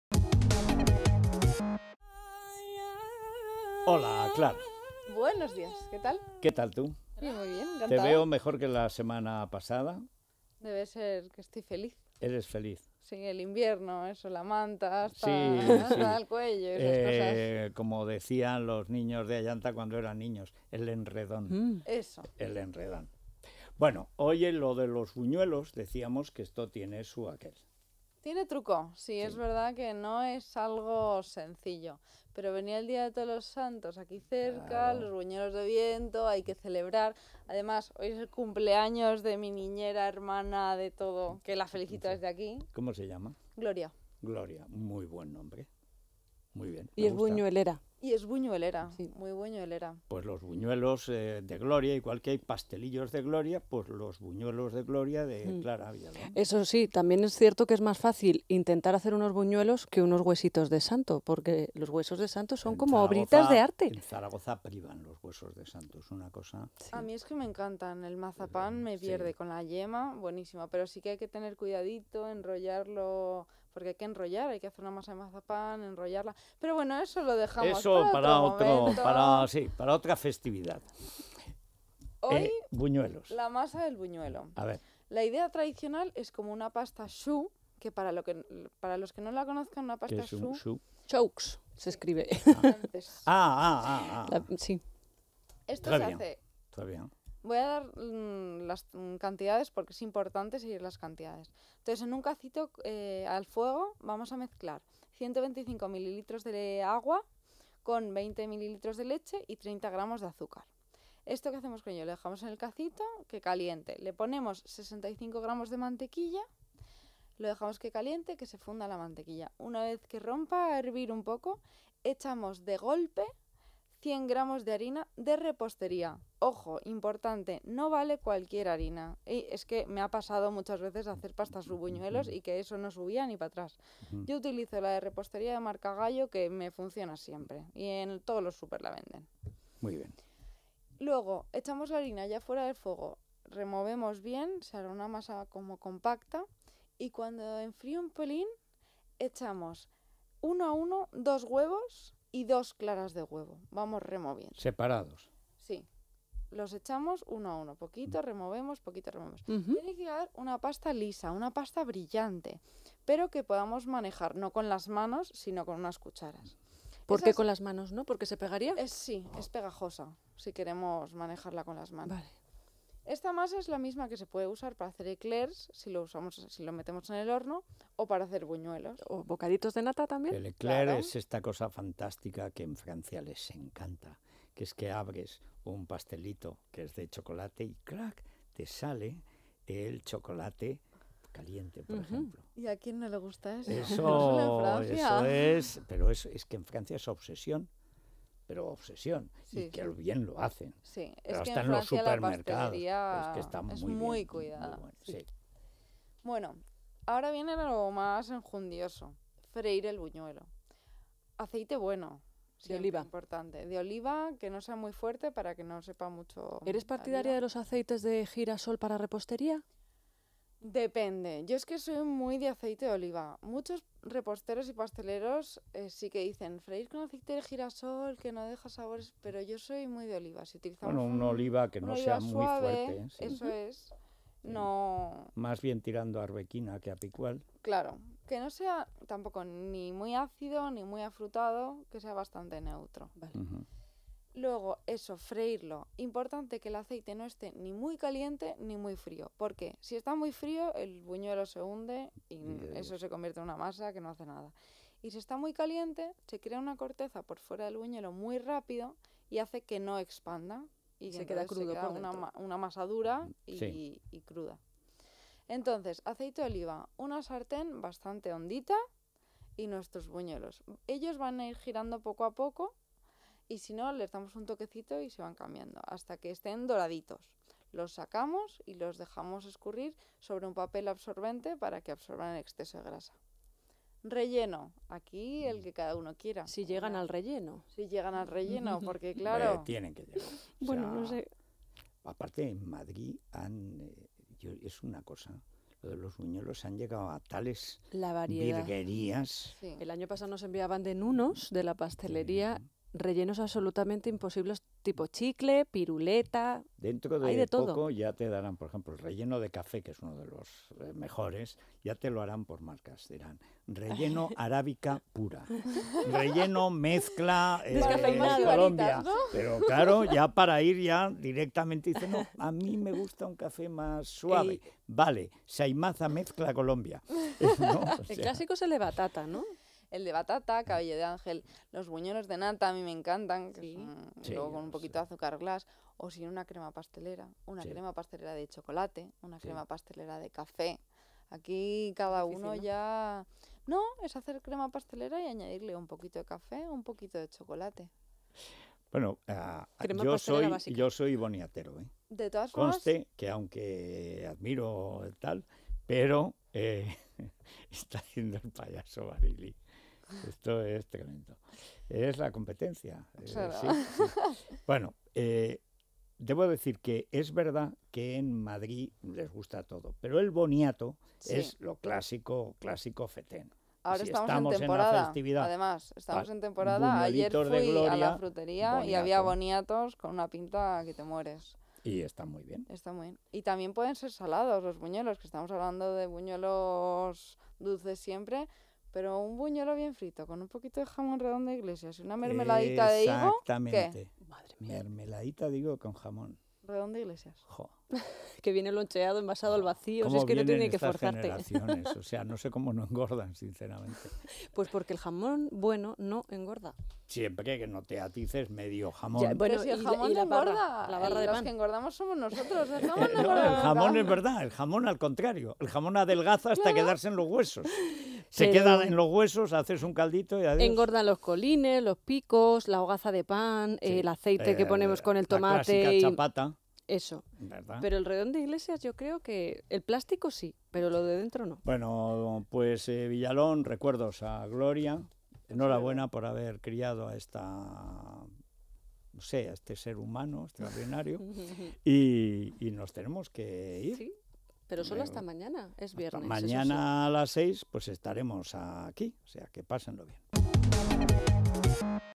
La Receta con Clara Villalón: Buñuelos - 24/10/13
Clara P. Villalón nos enseña a preparar buñuelos.